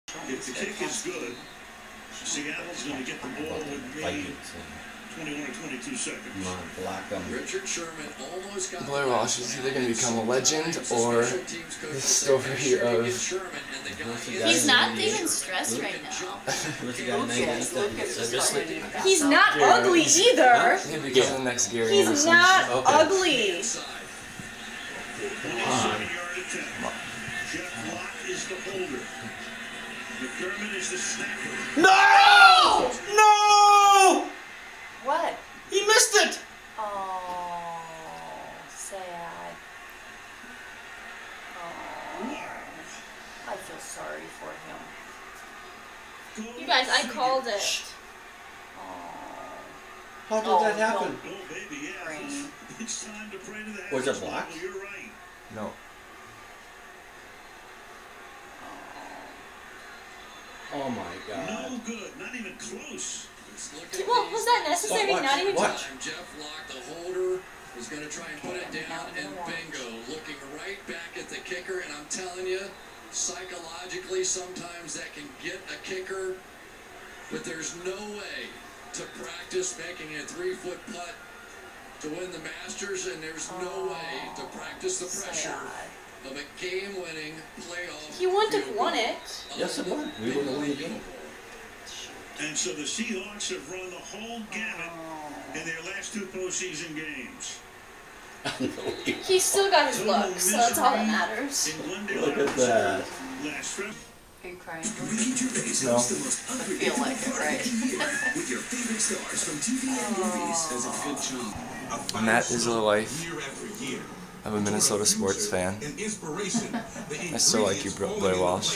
Blair Walsh Missed Field Goal Reaction
I set up the camera to catch our celebration. Instead this happened.